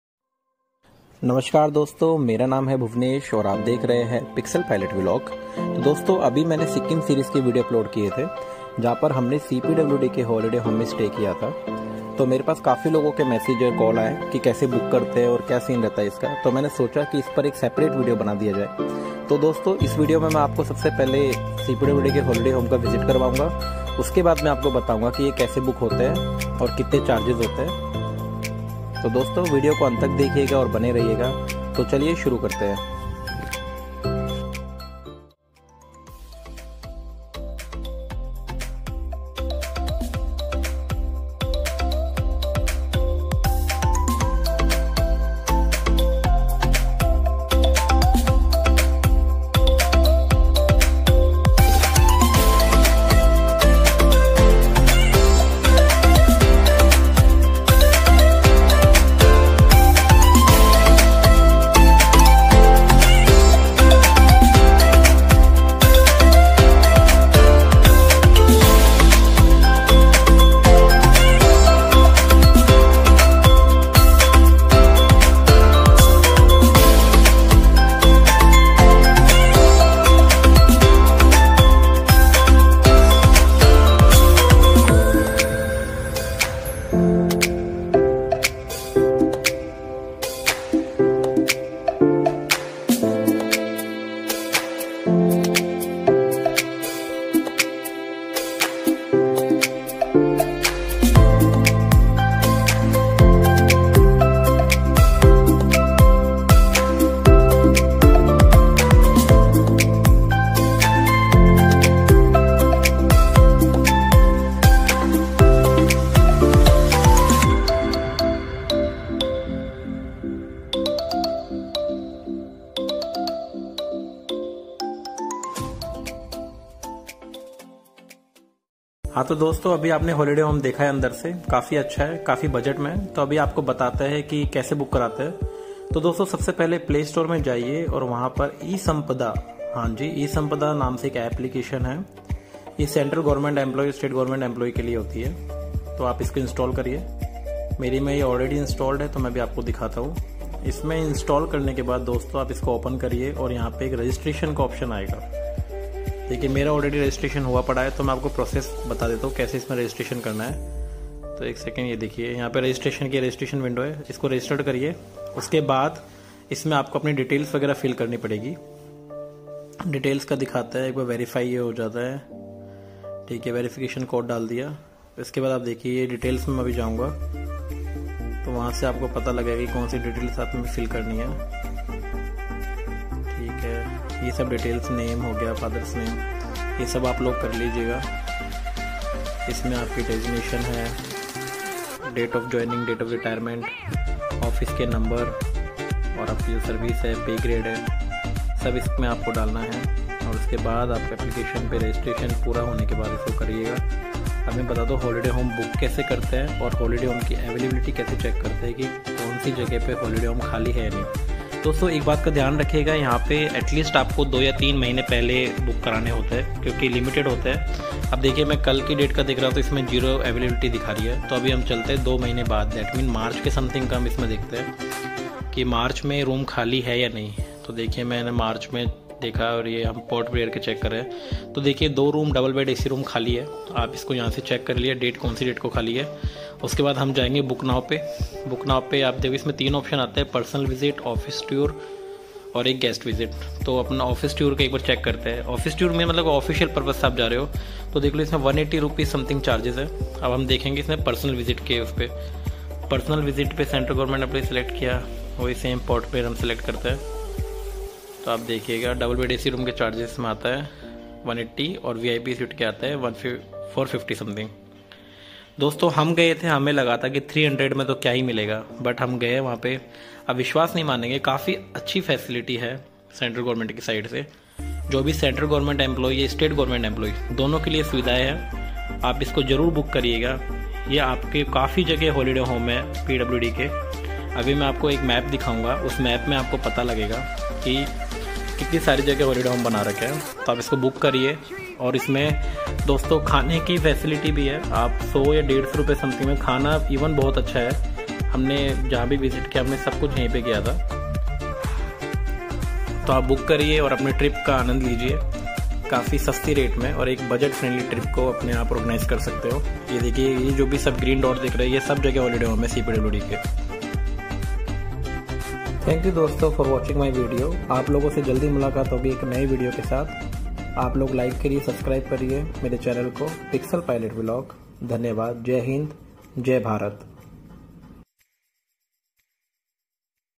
CPWD HOLIDAY HOME /कैसे बुक करे?/GANGTOK/CHARGES /300/- ONLY /DOUBLE BED AC ROOM/ESAMPADA PORTAL
Central Government Holiday Home Booking Online as per the new system introduced at esampada portal. Check the list of central govt guest houses in India. Book your accommodation in online with VIP suit room or Double / Four bed room in required guest house. Find the eligibility of employee and retired before booking…

How to Book Central Government Holiday Home Online

Presenting the easy guide for how to book a Central Government holiday home, check simple steps for easy booking

Open eSampada portal and Click Login

Open esampada.mohua.gov.in/signin/ and Tap on Login button

Provide Registered Mobile

Submit OTP and Sign In

Enter OTP received on mobile, Select Security Captcha and Click Sign In

Complete profile if not completed

For first login Users Click My Profile to complete your profile page

Click Book Now

Click on Holiday Homes / Guest House Booking on left side panel and Click Book Now

Select Visit Type

Select Personal Visit / Guest Visit or Official Tour and Upload id card details

Select Other details

Select Category for employee service, Holiday Home Location, Check In and Check Out Dates

Select Room Details

Select Room Type and Room Number, Click Make Payment and OK

Check the details and Confirm to complete the payment

Once payment has been completed, SMS will be sent to registered mobile number

Receive holiday home booking details

Receive the book details on your registered email and mobile number

This generated online slip will act as your proof of booking. The same online booking slip should submit to confirm Guest House while check in at Central Government holiday home. Also any payment made during online booking may not refund or cancel once the application booking confirmed.

music used in this vlog is by -